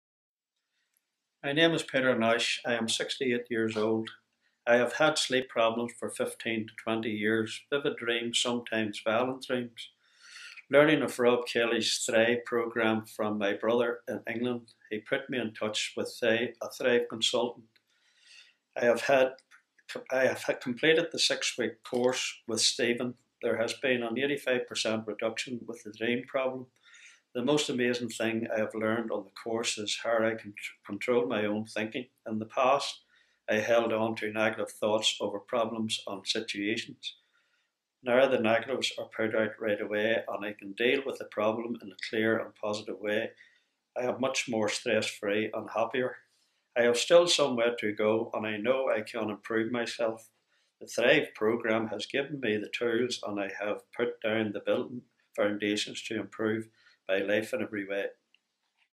"I have the tools to improve my life in every way!"
Around 30-40% of the general population have problems with sleeping and getting a full night's sleep, with about 10% reporting severe insomnia...

As Peter shows, it's never too late to make big, positive changes in life 😴🛌